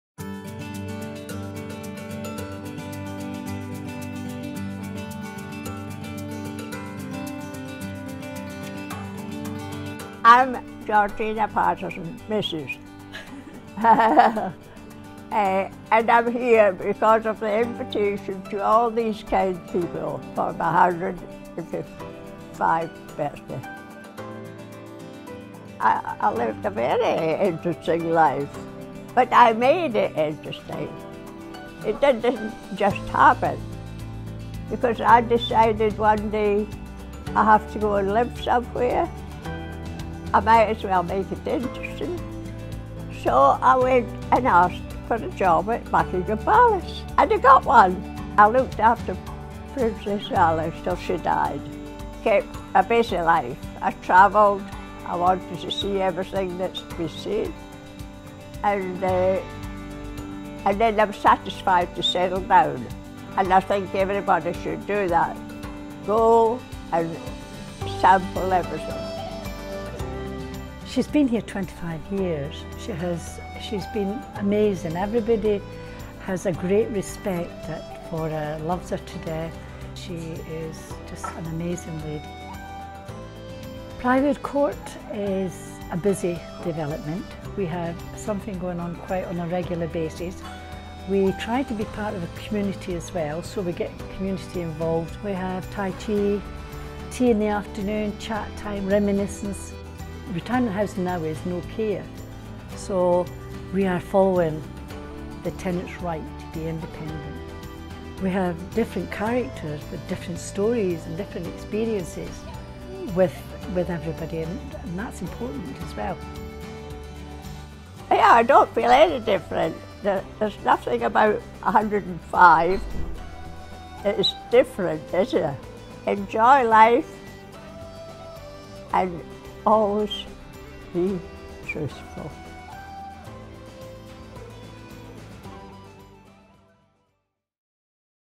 Georgina Paterson 105th Birthday
A remarkable Melrose resident showed that age has no limits as she danced the night away during her 105th birthday celebrations.

Georgina Paterson celebrated her incredible milestone birthday at Bield’s Priorwood Court in Melrose, surrounded by friends and family.